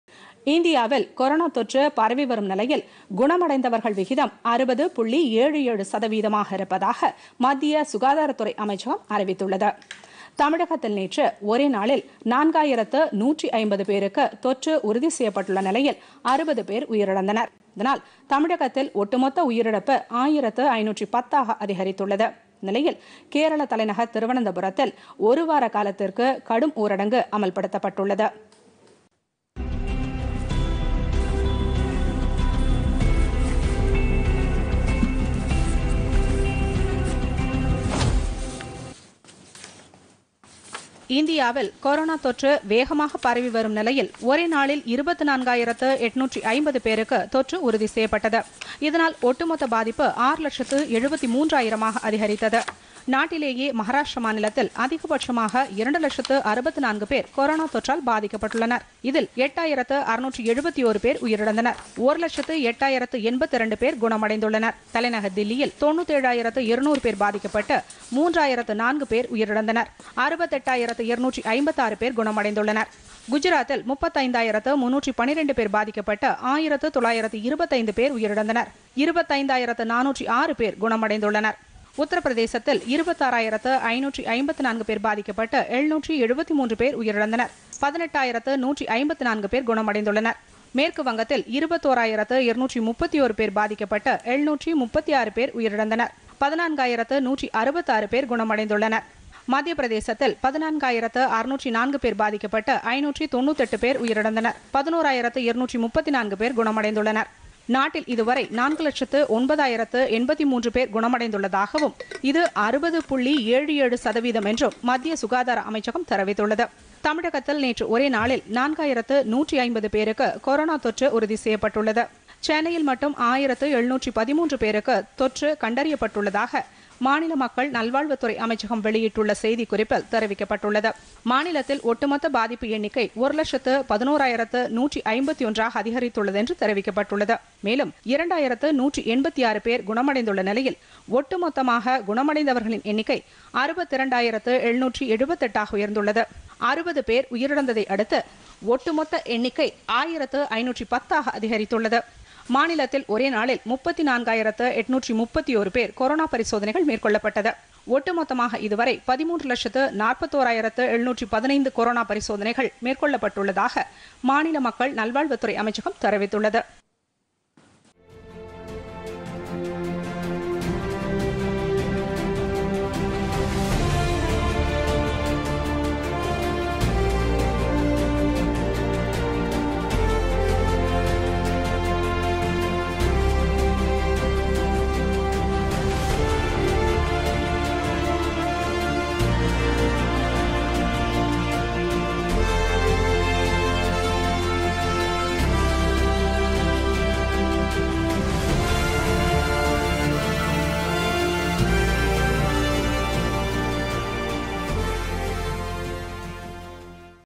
PodhigaiTamilNews பொதிகைசெய்திகள்
நாட்டில் கொரோனா தொற்றிலிருந்து குணமடைந்தோர் சதவீதம் 60 புள்ளி ஏழு ஏழாக அதிகரிப்பு - உலகளவில் தொற்று பாதிப்பில் மூன்றாவது இடத்திற்கு சென்றது இந்தியா.Visit : TamilNews - Doordarshan